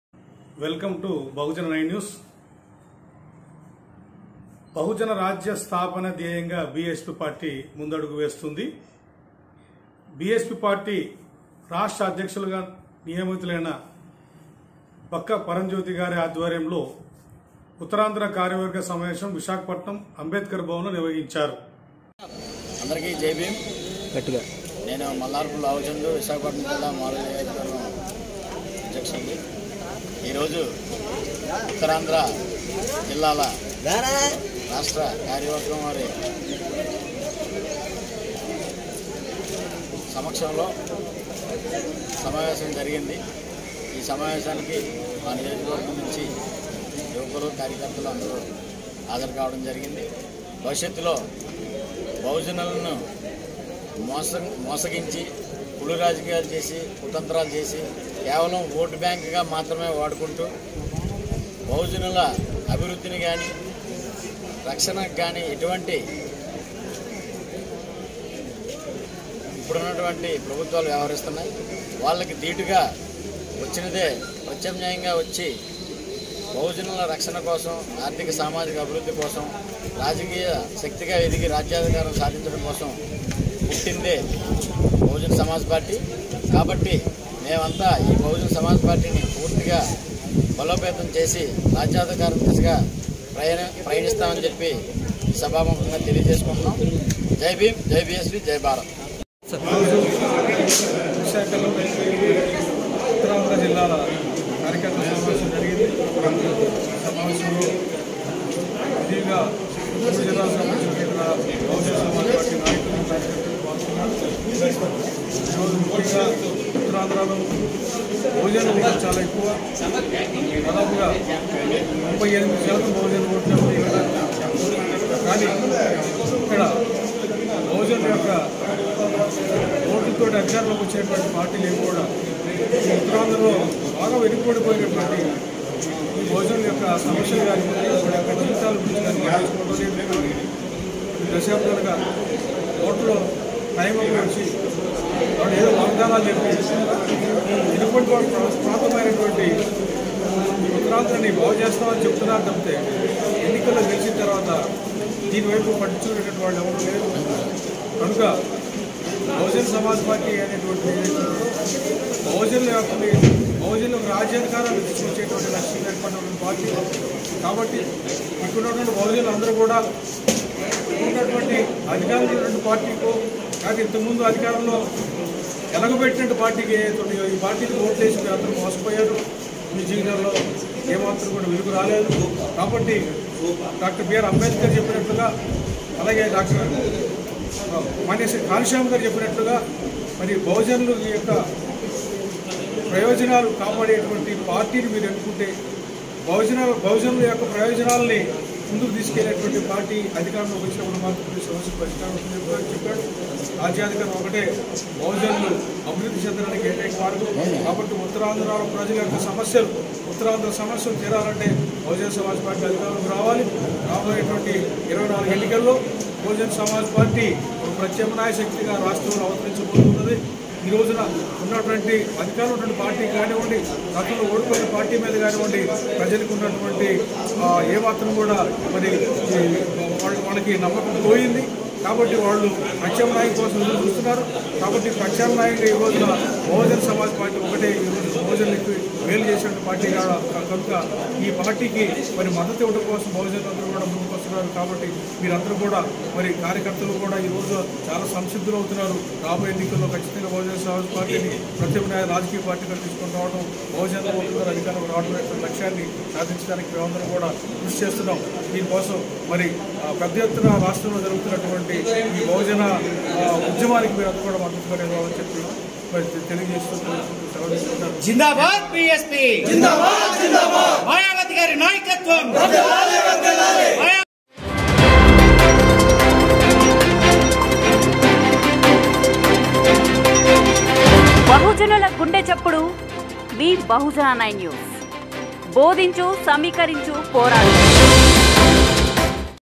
బహుజన రాజ్య స్థాపనే ద్యేయం గా "BSP" పార్టీ కార్యాచరణ.
BSP పార్టీ  రాష్ట్ర అధ్యక్షులు గా నూతనంగా నియమితులైన ,
"బక్క పరంజ్యోతి" గారి ఆధ్వర్యంలో, ఉత్తరాంధ్ర కార్యవర్గ సమావేశం, విశాఖపట్నం అంబేద్కర్ భవన్ లో నిర్వహించారు...

2024 అసెంబ్లీ ఎన్నికలే ద్యేయంగా ఇప్పటి నుండే పార్టీ కోసం కష్టపడి, పార్టీని బలోపేతం చేయాలని కోరారు...
ఇప్పుడున్న ప్రభుత్వం ,బడుగు బలహీన వర్గాలు పై జరుగుతున్న దాడులపై మొద్దునిద్ర వహిస్తుందని ఎద్దేవా చేసారు.
బహుజనులుకు రక్షణ కేవలం "బహుజన సమాజ్ పార్టీ"తోనే సాధ్యమని తెలిపారు.
ఈ కార్యక్రమంలో ఉత్తరాంధ్ర నాయకులు, కార్యకర్తలు పాల్గొన్నారు.